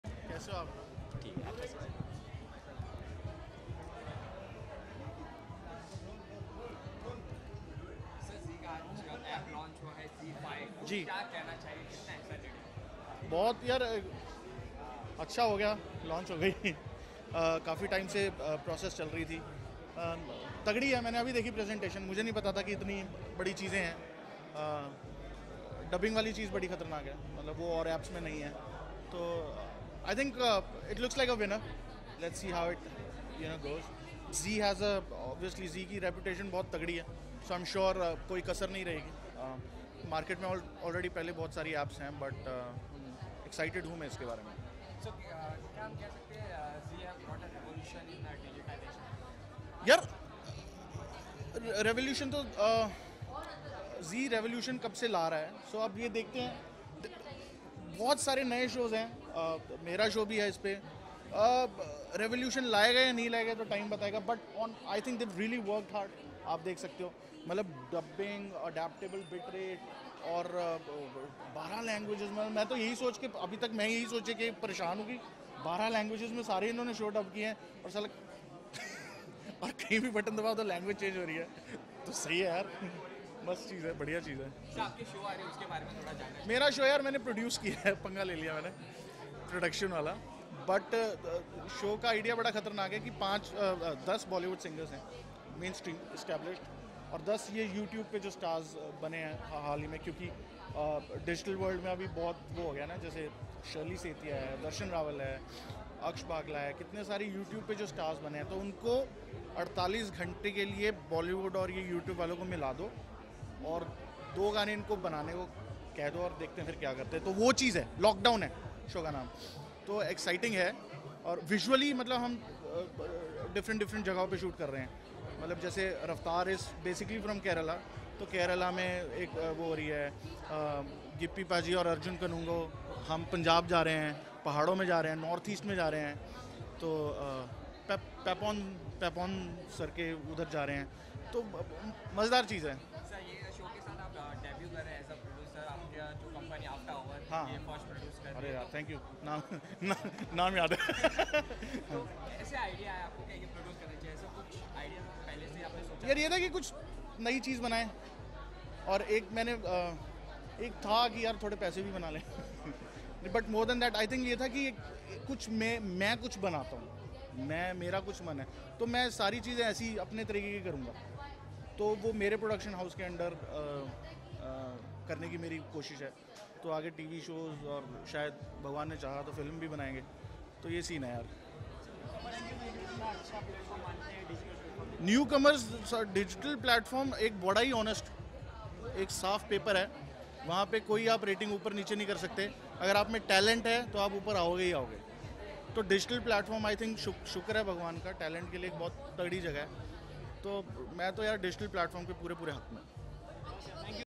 Badshah Talk About His Upcoming Show | Badshah Full Interview | ZEE5 Launch | Zee Tv 2018
Badshah Talk About His Upcoming Show Badshah Full Interview ZEE5 Launch Zee Tv 2018 Bollywood Latest News Movies Songs 2018